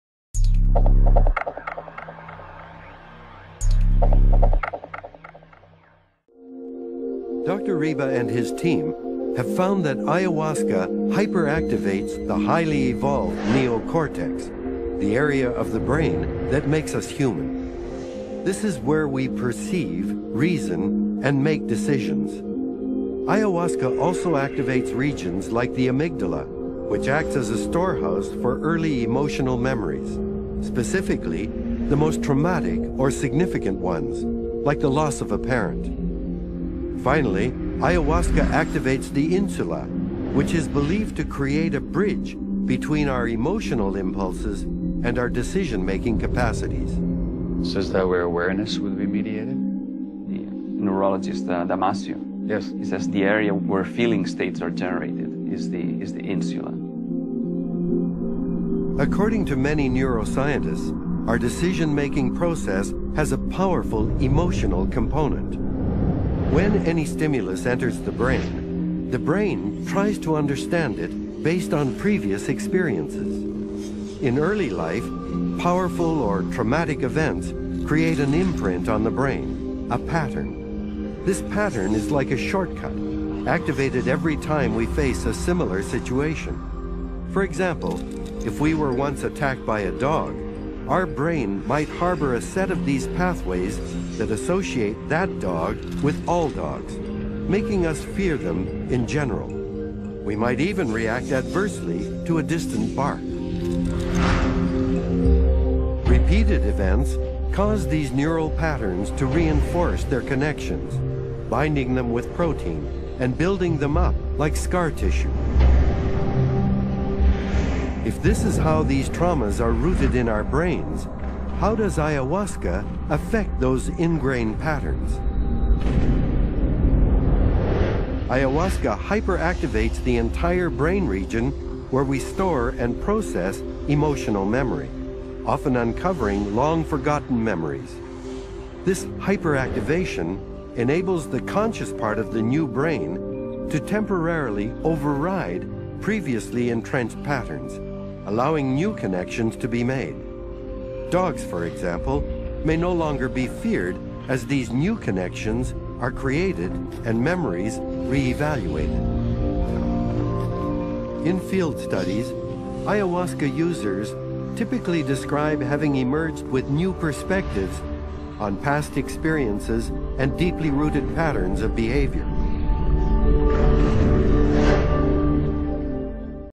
How does Ayahuasca work? كيف تتفاعل آياهواسكا مع الدماغ؟ (AR SUBS)
How does Ayahuasca affect traumatic events ingrained in our brain?

Ayahuasca hyperactivates the highly evolved neocortex. The area of the brain that makes us human. This is where we perceive reason and make decisions. Ayhauasca also activates regions like the amygdala; which acts as a storehouse for early emotional memories, specifically the most traumatic or significant ones. Like the loss of a parent.

This short video provides an explanation of the health benefits of Ayahuasca from a scientific perspective.

كيف يؤثر آياهواسكا على الأحداث المؤلمة المتأصلة في دماغنا؟

آياهواسكا تنشط القشرة المخية الحديثة المتطورة. منطقة الدماغ التي تجعلنا بشرا. هذا هو المكان الذي ندرك فيه المنطق ونتخذ القرارات. تنشط أيهاواسكا أيضًا مناطق مثل اللوزة. والذي يعمل كمخزن للذكريات العاطفية المبكرة ، وتحديدا أكثرها ألما أو أهمية. مثل فقدان أحد الوالدين.

يقدم هذا الفيديو القصير شرحًا للفوائد الصحية لآياواسكا من منظور علمي.

Love & Light
ⵍoⵯⴻ & ⵍⵉⴳⵀⵜ